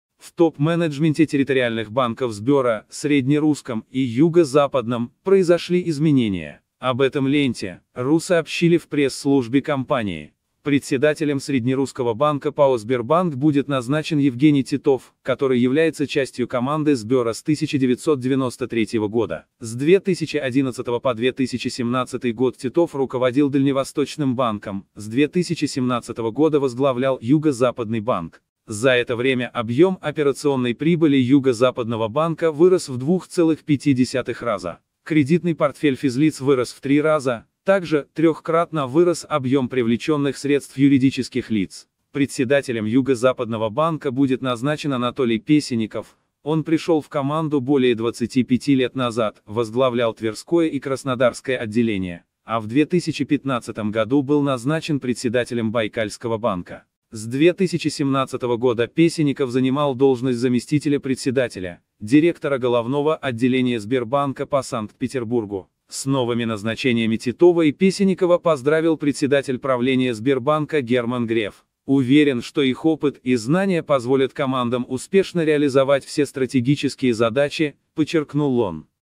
В топ-менеджменте территориальных банков Сбера произошли изменения
В топ-менеджменте территориальных банков Сбера — Среднерусском и Юго-Западном — произошли изменения. Об этом «Ленте.ру» сообщили в пресс-службе компании.

Председателем Среднерусского банка ПАО Сбербанк будет назначен Евгений Титов, который является частью команды Сбера с 1993 года. С 2011 по 2017 год Титов руководил Дальневосточным банком, с 2017 года возглавлял Юго-Западный банк. За это время объем операционной прибыли Юго-Западного банка вырос в 2,5 раза. Кредитный портфель физлиц вырос в три раза, так же — трехкратно — вырос объем привлеченных средств юридических лиц.

Председателем Юго-Западного банка будет назначен Анатолий Песенников — он пришел в команду более 25 лет назад, возглавлял Тверское и Краснодарское отделения, а в 2015 году был назначен председателем Байкальского банка. С 2017 года Песенников занимал должность заместителя председателя, директора Головного отделения Сбербанка по Санкт-Петербургу.

С новыми назначениями Титова и Песенникова поздравил председатель правления Сбербанка Герман Греф. «Уверен, что их опыт и знания позволят командам успешно реализовать все стратегические задачи», — подчеркнул он.